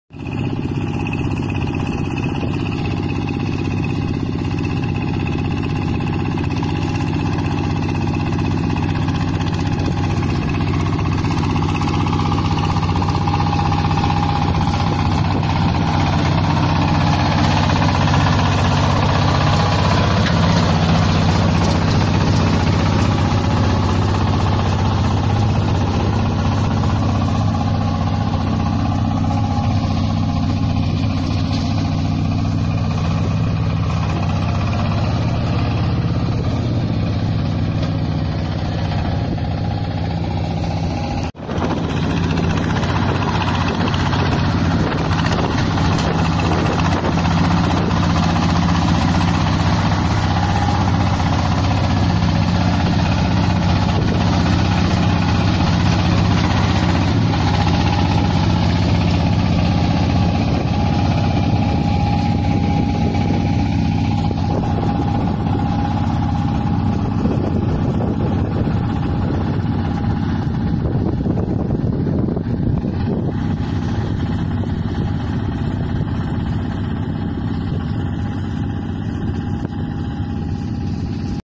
เรือไถนา เฟื่องฟ้า
เรือไถนา เฟื่องฟ้า สุพรรณบุรี
เหมาะสำหรับนาลุ่มนาหล่ม และ นาแห้งนาดอน
ขับขี่สบายใช้งานสะดวก ราคา 138,000